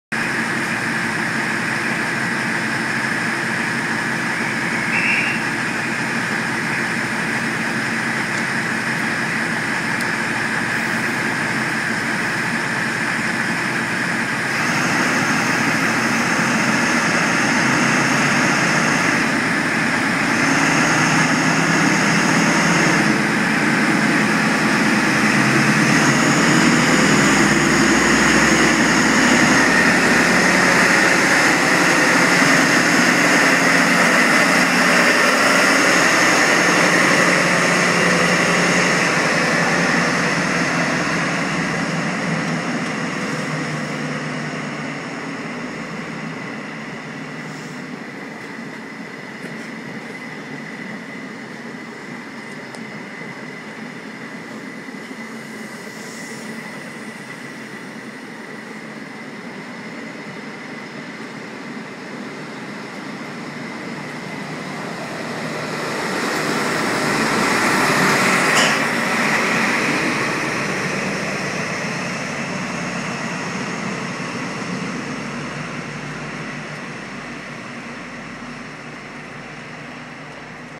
East midlands trains 43054 and 43073
East midlands trains 43073 and 43054 leaving leicester